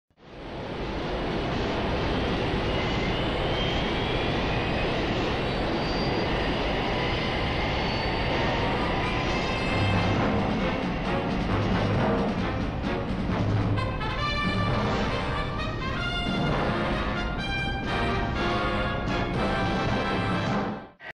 Space X
Space X fake reverse landings and NASA fake space travel